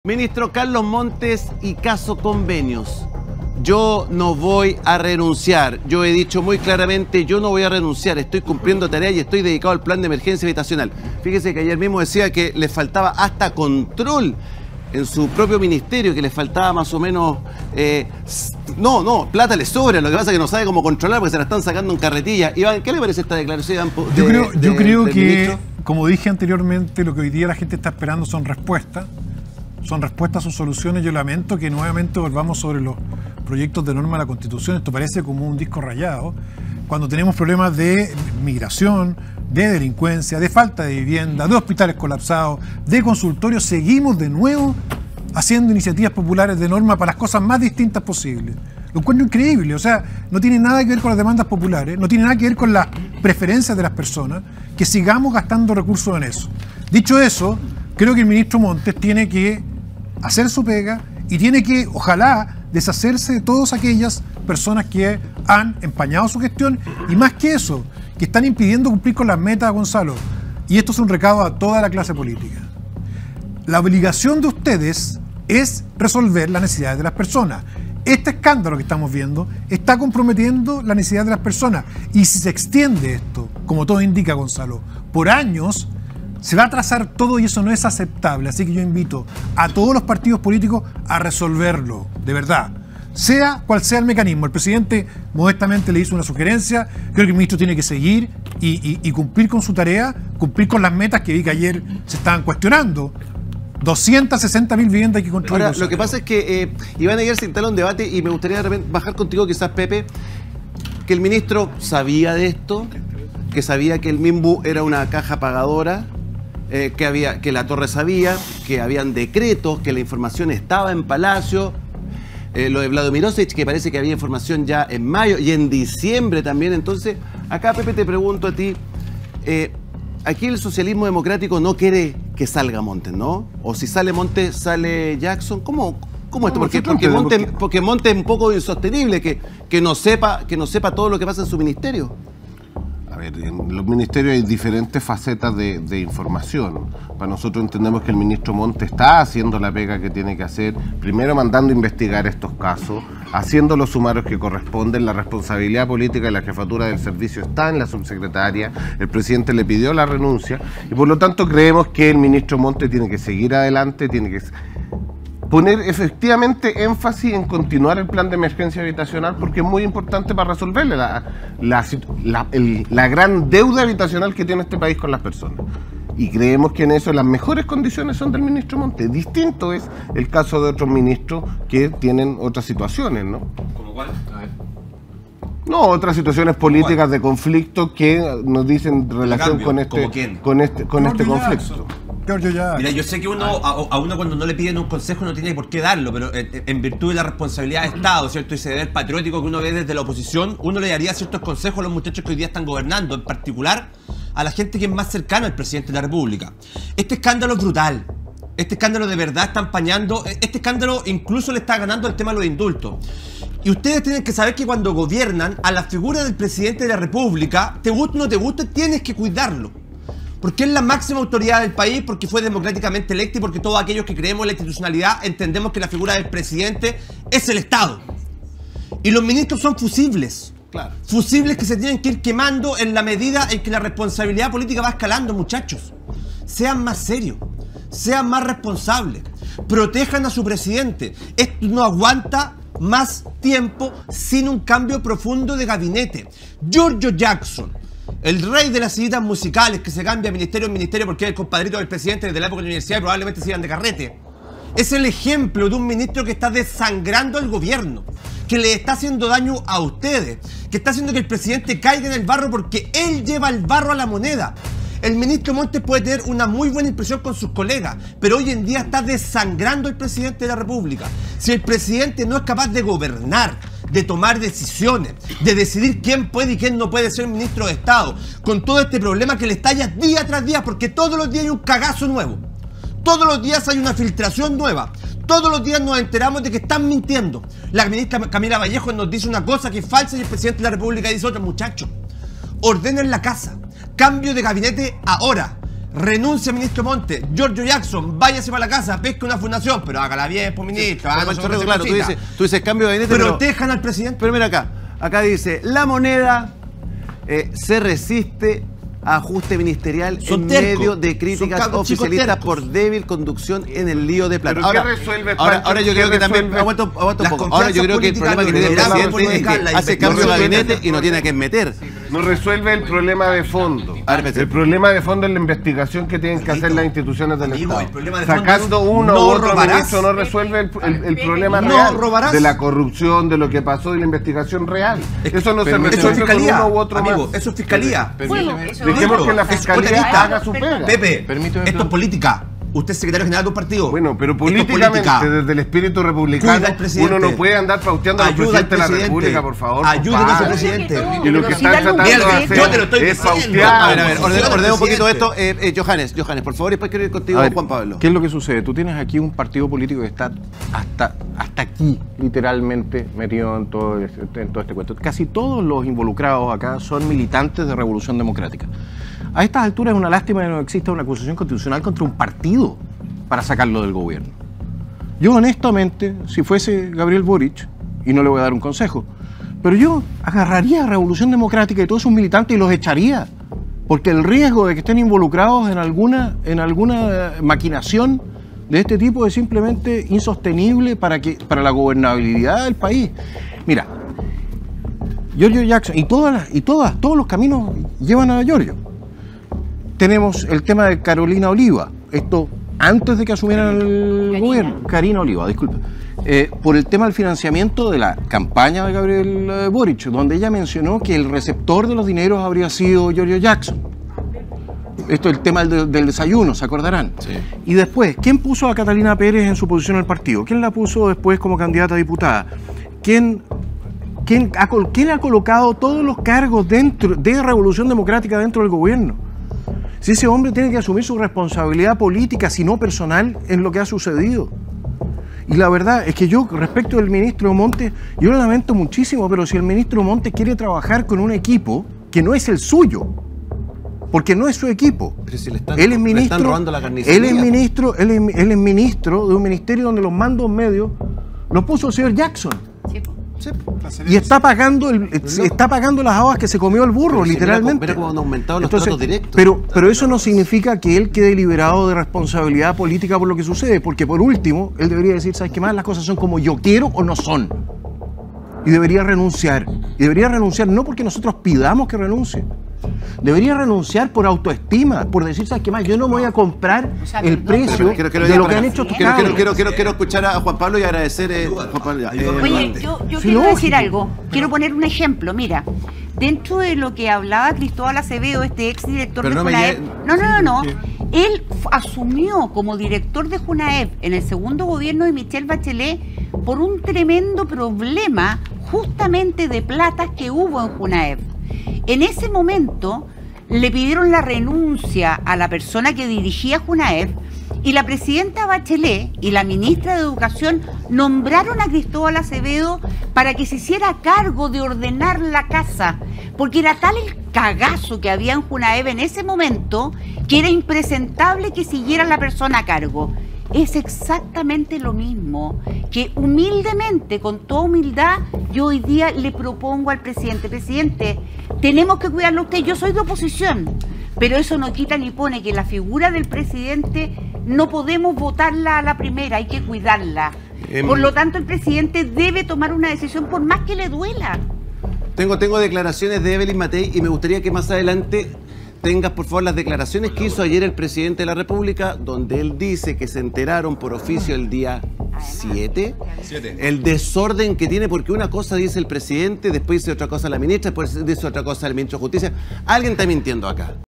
MINISTRO NO QUIERE RENUNCIAR luego de IRREGULARIDADES por FUNDACIONES
¡Alta tensión! 🔥 En el episodio de hoy los panelistas: Francisco Orrego, Dauno Totoro, Gloria Naveillán, Juan Pablo Arriaza, Iván Poduje, Alejandro Aguilera, Johannes Kaiser y José Toro analizan y debaten la contingencia. 🇨🇱 Prepárate para un acalorado cruce de ideas.

¿Estás listo/a para un debate con alta tensión? 🥊 Nuestros panelistas desgranan los temas más importantes del día:  El ministro de Vivienda, Carlos Montes, rechaza renunciar a su cargo a pesar del escándalo por el Caso Convenios y la confusión sobre la fecha en que La Moneda tuvo conocimiento de los antecedentes. Montes se compromete a colaborar para esclarecer las irregularidades y mejorar el sistema, evitando entrar en polémicas sobre la fecha de notificación. Su enfoque principal es el plan de emergencia habitacional y trabajar en la resolución de los problemas del sistema.